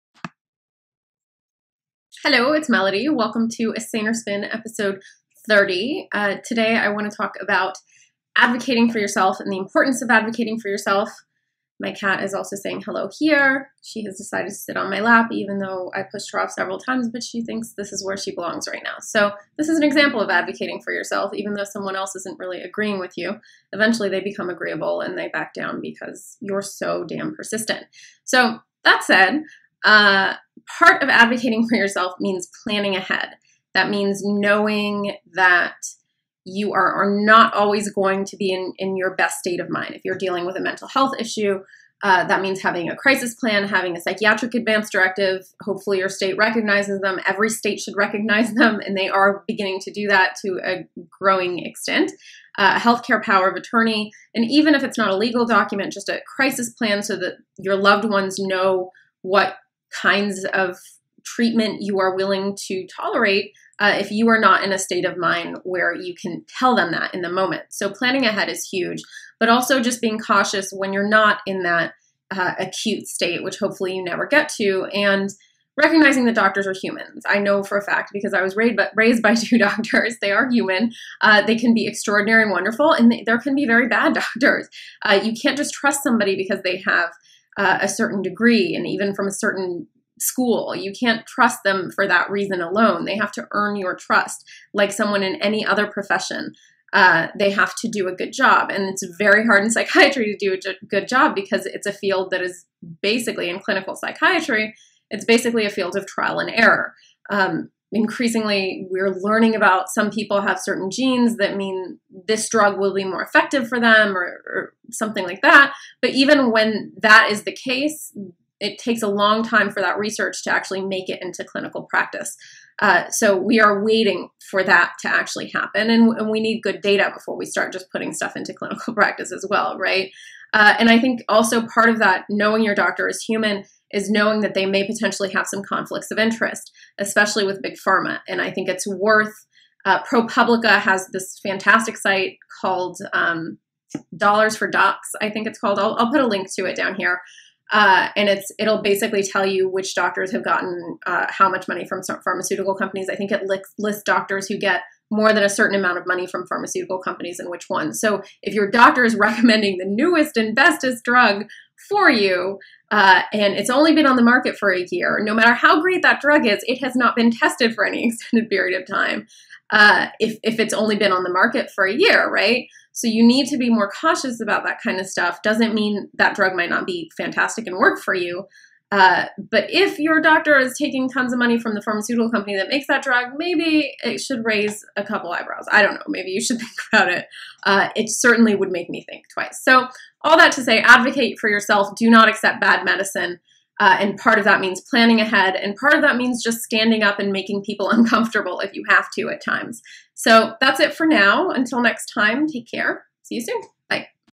A Saner Spin, Ep. 30: How to Be Your Own Advocate
Some tips on how to be your own best advocate as someone living with a mental health condition. Here is a link to the ProPublica Dollars for Docs resource that I mention in the clip